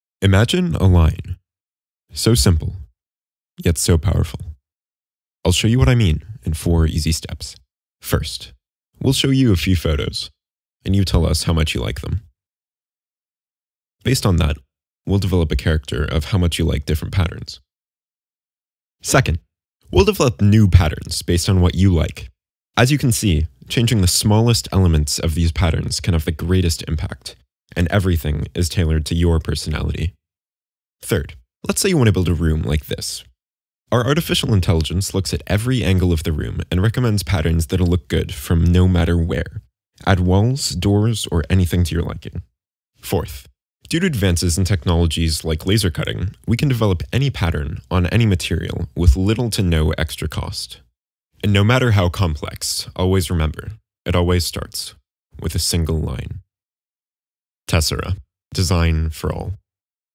Accepted USC Iovine and Young Academy Video Proposal 2020
Video proposal by Will Sturgeon. Accepted by IYA.